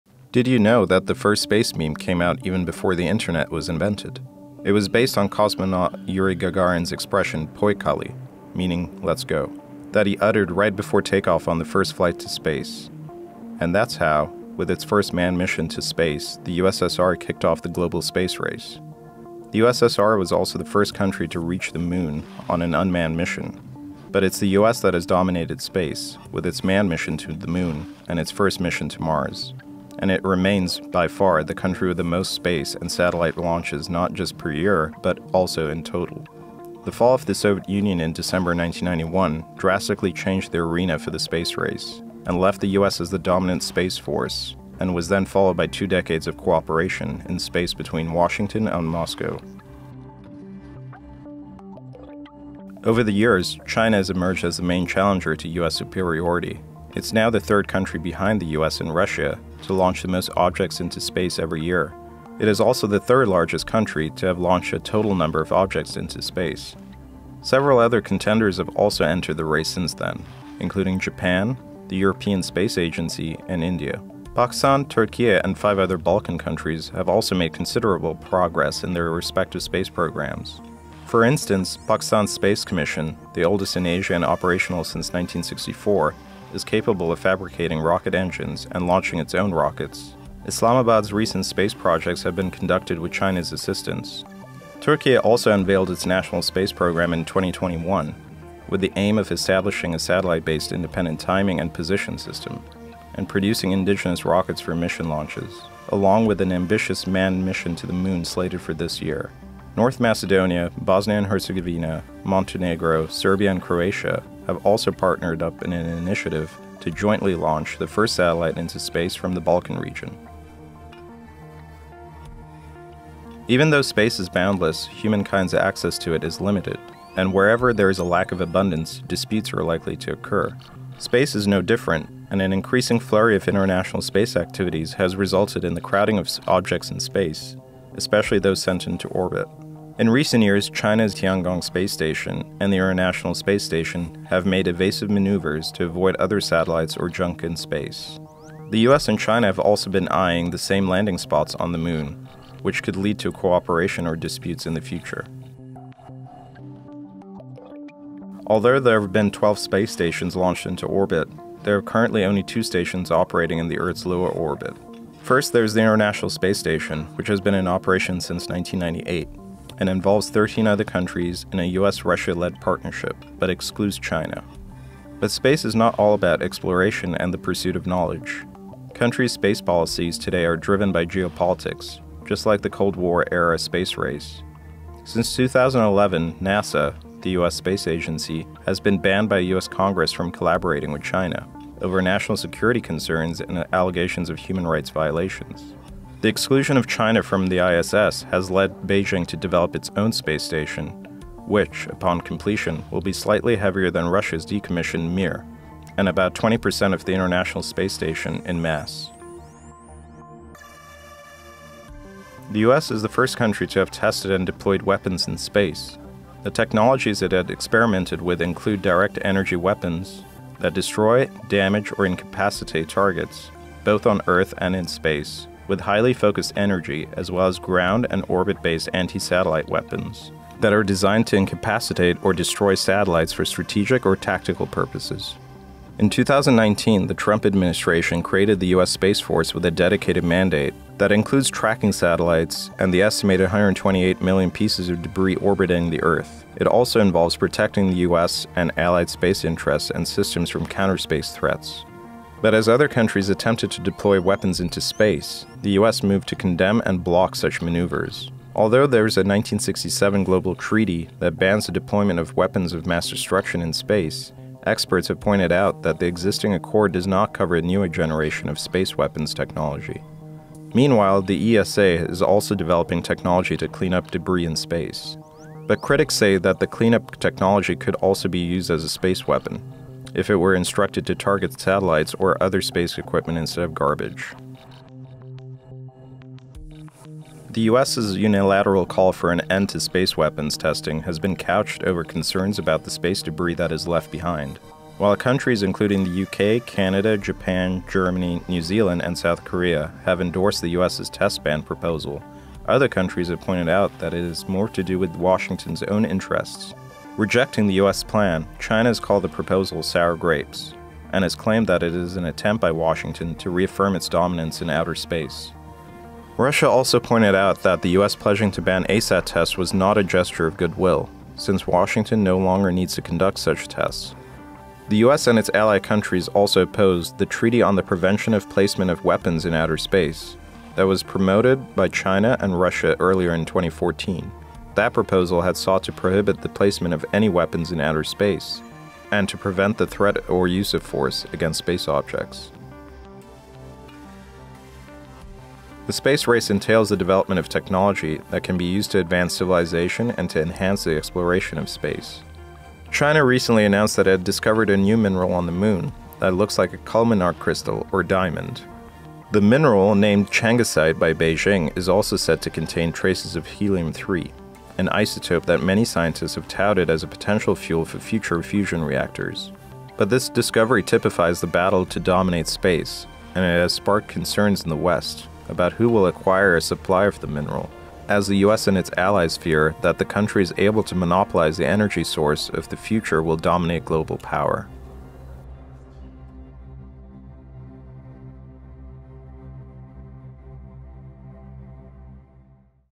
The race for space: What does it look like?
After the addition of new players, technology, and politics, here is a glimpse at what the Space Race looks like in the 21st century.